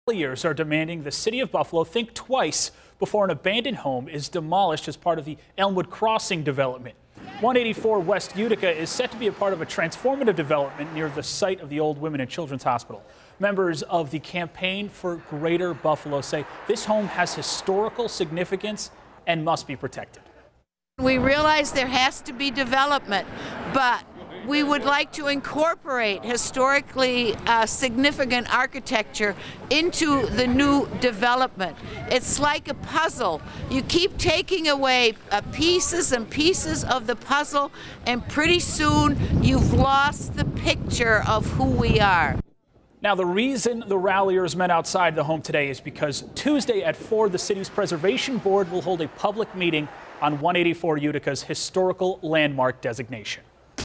Neighbors fight to save 184 W Utica from demolition
184 West Utica is set to be a part of a transformative development near the site of the old Women and Children's Hospital. Members with the campaign for Greater Buffalo say this home has historical significance and must protected.